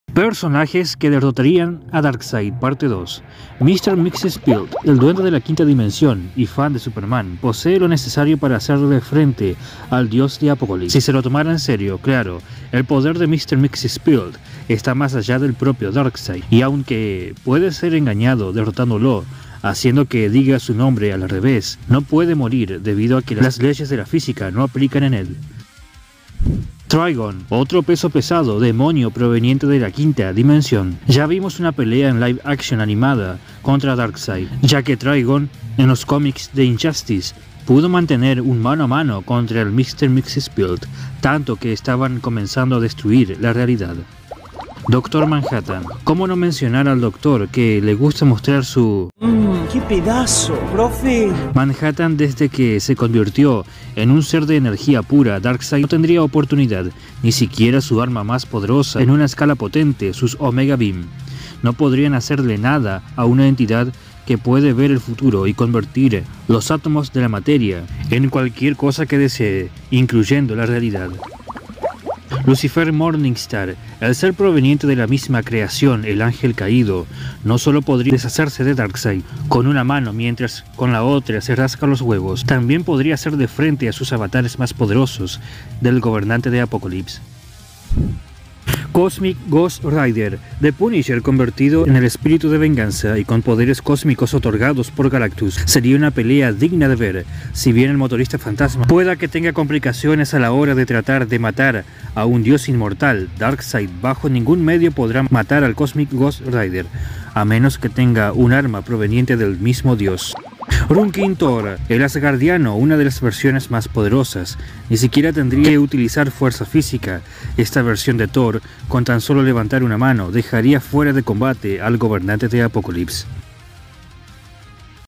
PERSONAJES QUE DERROTARIAN A DARKSEID PT 2
segunda parte de personajes que le harían frente al gobernante de apokolips y nuevo Dios Darkseid